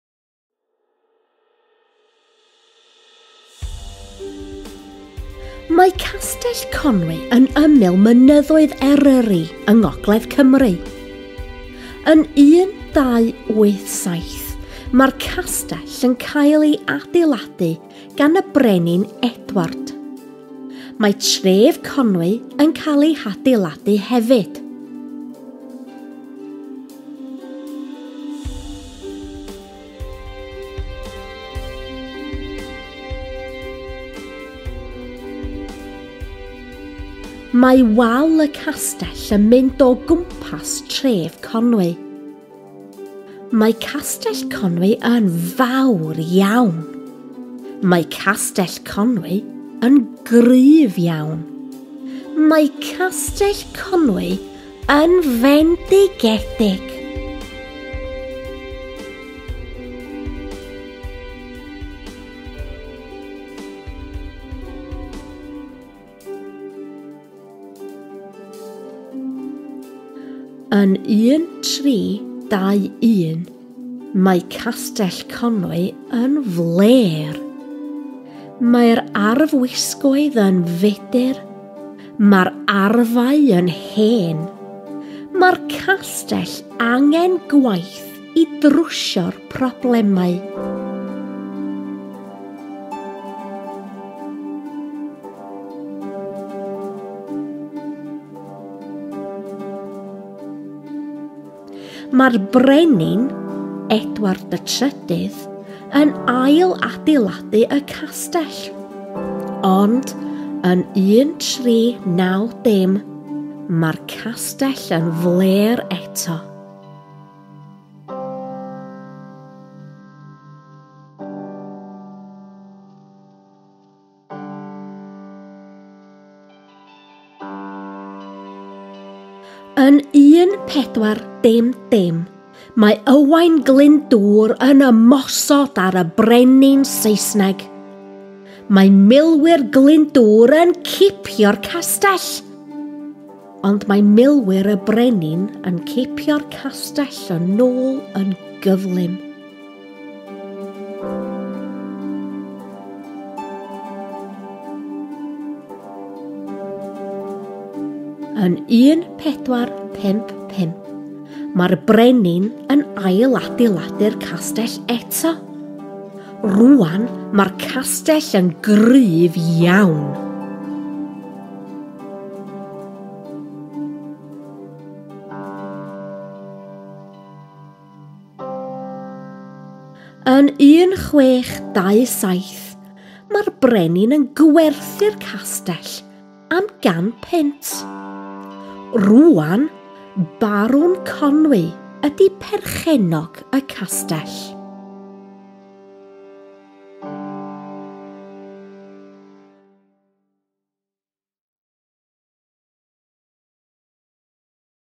M1.4 Castell Conwy II
Mae Castell Conwy yn ymyl mynyddoedd Eryri yng Ngogledd Cymru. Yn 1287 mae’r castell yn cael ei adeiladu gan y Brenin Edward. Mae tref Conwy yn cael ei hadeiladu hefyd. Mae wal y castell yn mynd o gwmpas tref Conwy. Mae castell Conwy yn fawr iawn. Mae castell Conwy yn gryf iawn. Mae castell Conwy yn fendigedig.
Yn 1321 mae castell Conwy yn fler. Mae’r arfwisgoedd yn fudur. Mae’r arfau yn hen. Mae’r castell angen gwaith i drwsio’r problemau. Mae’r brenin, Edward III, yn ail-adeiladu y castell, ond yn 1390 mae’r castell yn fler eto.
Yn 1400 mae Owain Glyndŵr yn ymosod ar y brenin Saesneg. Mae milwyr Glyndŵr yn cipio’r castell! Ond mae milwyr y brenin yn cipio’r castell yn ôl yn gyflym. Yn 1455 mae’r brenin yn ail-adeiladu’r castell eto. Rŵan mae’r castell yn gryf iawn.
Yn 1627 mae’r brenin yn gwerthu’r castell am £100. Rŵan, Barwn Conwy ydy perchennog y castell.